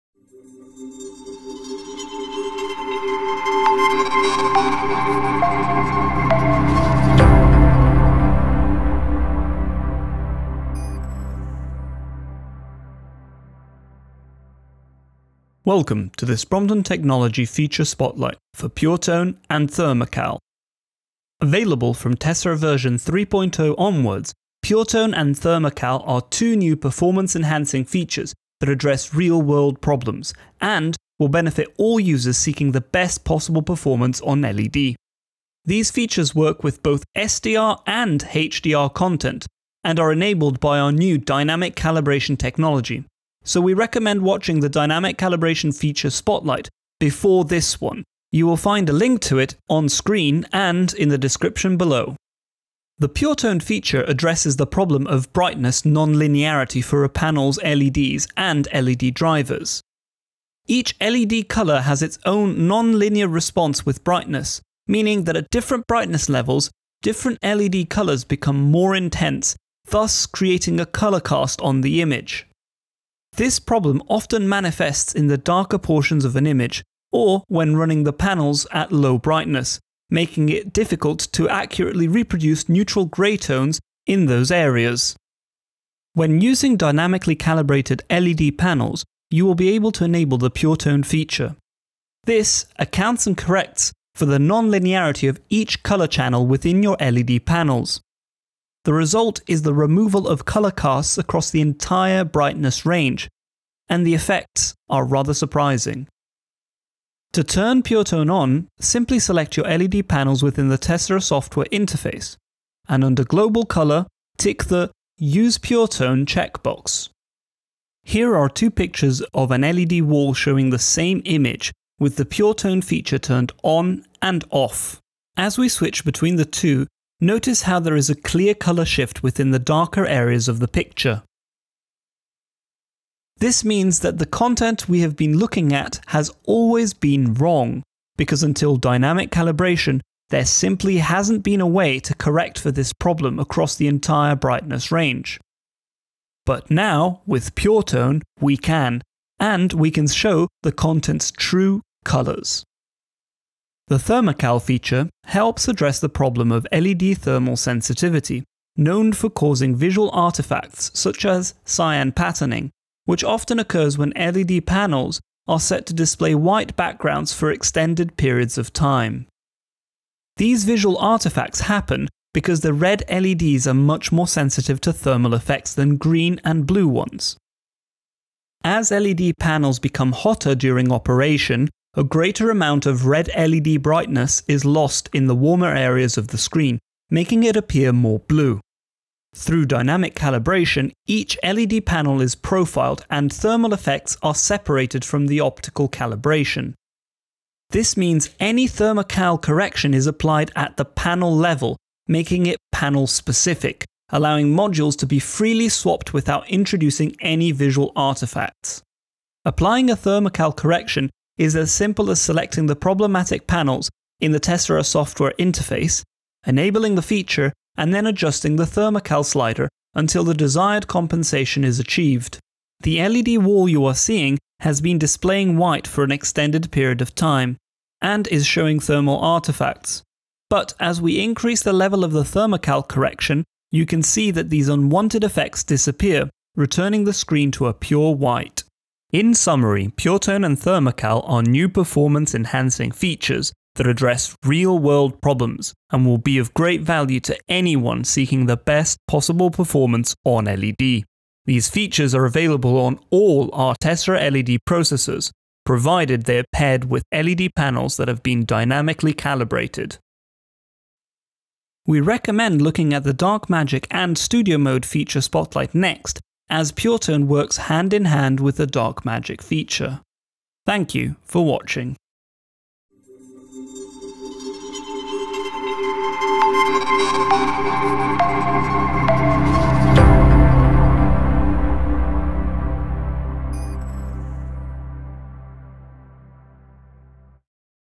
Feature Spotlight: PureTone and ThermaCal
Discover our performance enhancing features PureTone and ThermaCal. These benefit all users using both SDR and HDR content, and are enabled by our Dynamic Calibration technology.

Dark Magic and Studio Mode - TO BE ADDED

VIDEO TIMESTAMPS
0:00 - Opening
0:14 - Introduction
0:52 - PureTone Explained
1:46 - PureTone In Action
2:33 - ThermaCal Explained
3:29 - ThermaCal In Action
3:57 - Feature Summary
4:21 - Conclusion
4:32 - Outro